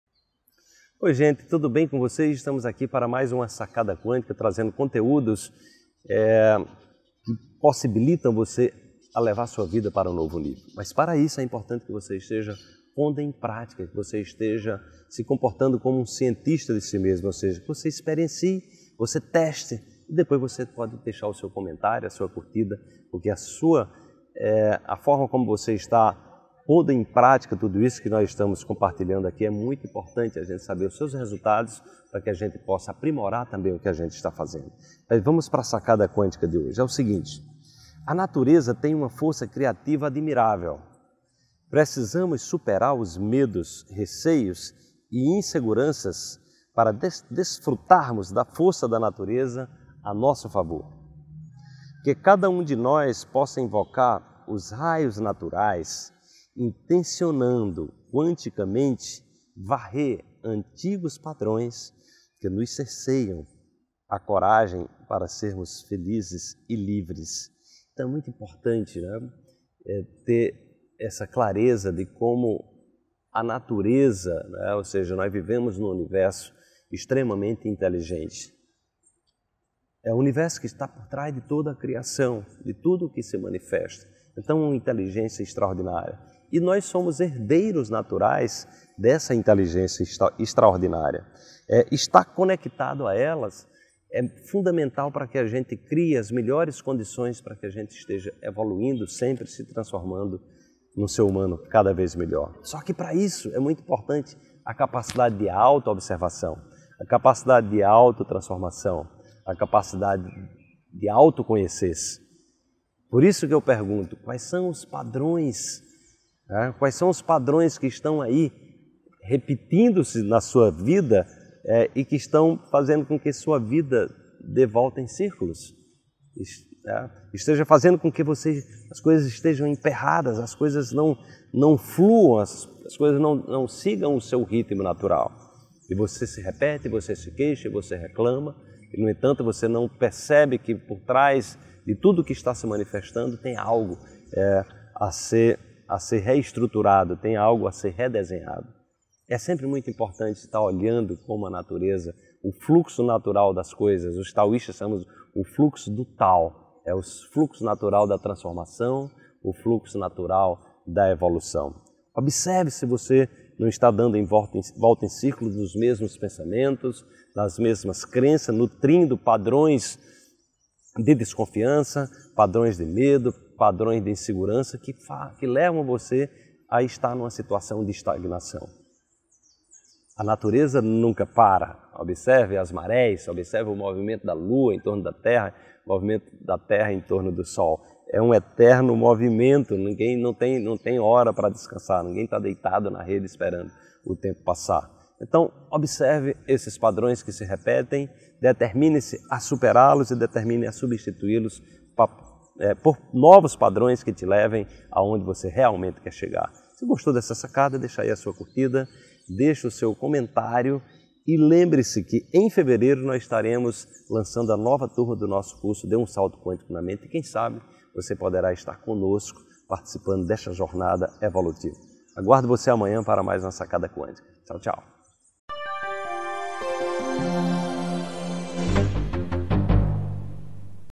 Coaching Quântico 239: A Chave Para Invocar a Força da Natureza a Seu Favor | Wallace Liimaa
✭✭✭✭✭ Nesse vídeo apresento dicas sobre como utilizar as forças e os recursos naturais ao nosso favor.  Assista já!